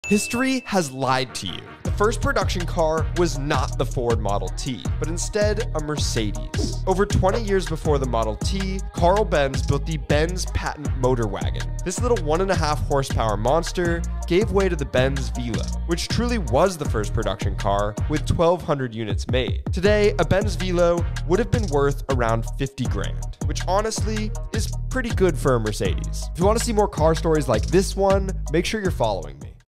The First Car Ever Made...
History seems to always be telling us that the Model T was truly the first real production car. While it may have been the most accessible to the everyday person the first production car was actually a Mercedes and it came much earlier then you might think...